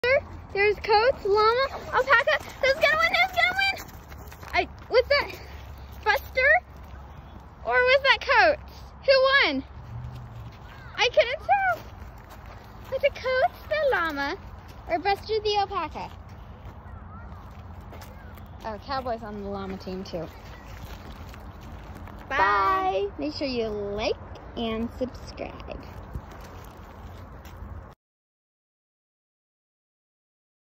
Llama vs alpaca race!
See one of llamas race our alpaca who wins?!